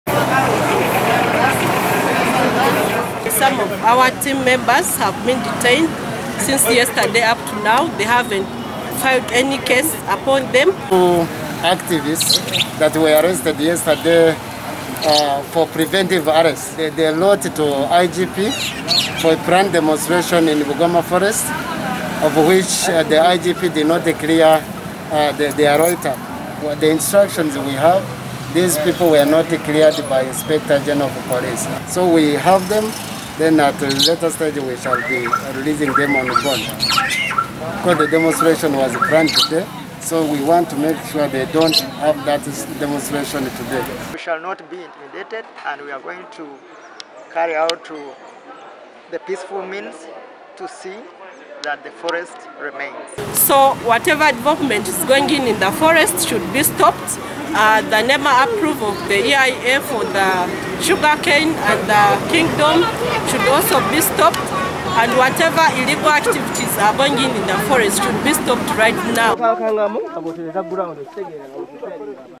OKUTAASA EKIBIRA BUGOMA: Poliisi ekutte bannamawulire babiri
E Hoima bannamawulire babiri poliisi ebakutte lwa kukunga abantu abalumirirwa obutonde bw'ensi okubeegattako okutaasa ekibira ky’e Bugoma ab’obuyinza kye baagala okuwaayo eri kampuni ya Hoima Sugar ekisaanyeewo oluvannyuma erimireko  ebikajjo. Venix Watebwa ne Joshua Mutale   ab’ekibiina  ky'obwannakyewa ki Water and Environment Network be bakwatiddwa.